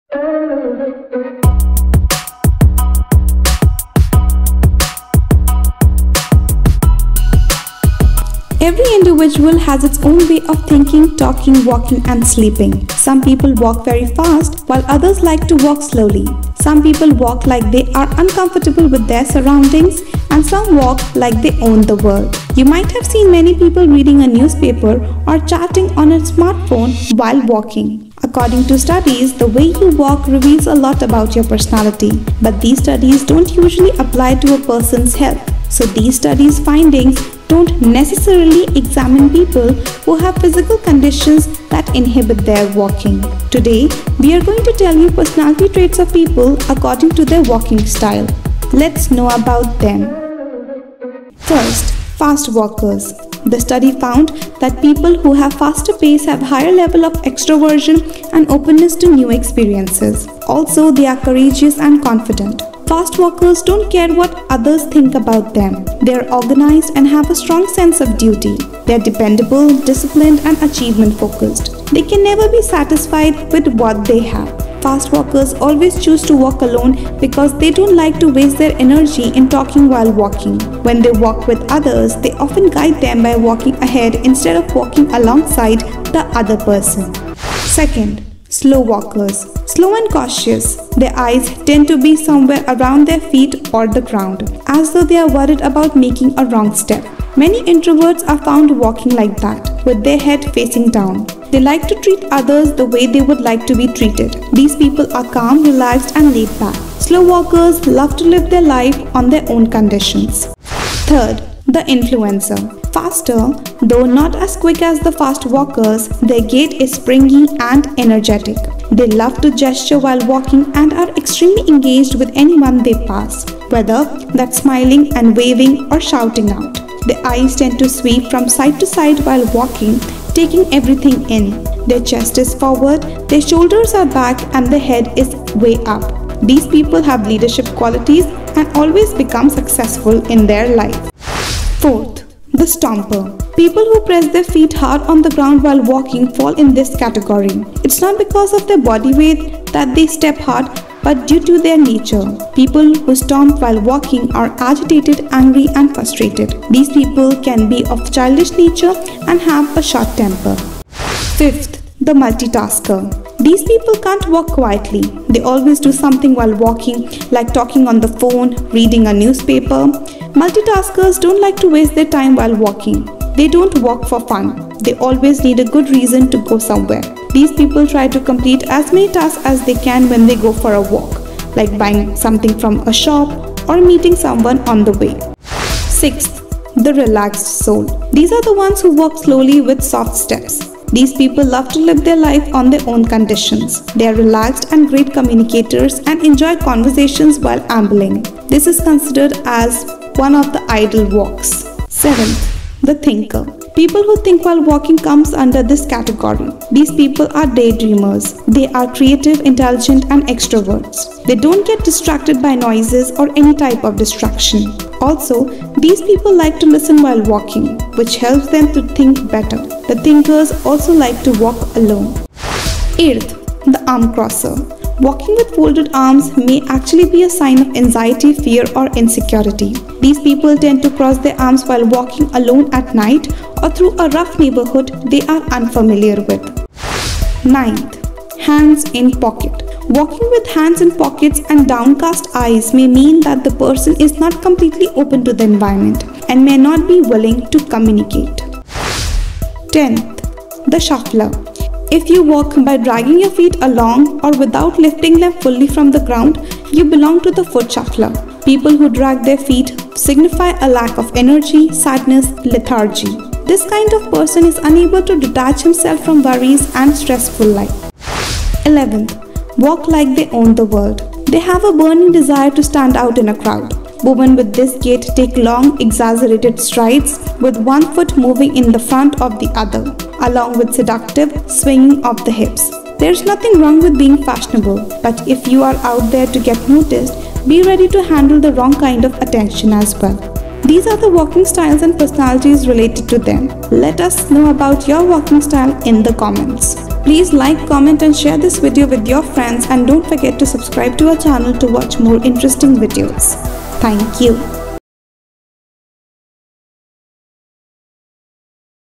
Your Walking Style Reveals Your Personality, Career & Lifestyle | The Magical Indian
Every individual has it's own way of walking, talking, sleeping and sitting. According to studies your walking style may tell a lot about your character and personality. Some people walk slow while others like to walk fast. Businessmen walk confidently and leaders gives different kinds of gestures while walking. Walking style also tell a lot about a woman's personality. It also tells about your mental condition. Some people don't like to waste their time so they work while walking that's why they are called as multitaskers.

Tell us about your walking style in comments.

 Know more about walking style and personality traits related to them in video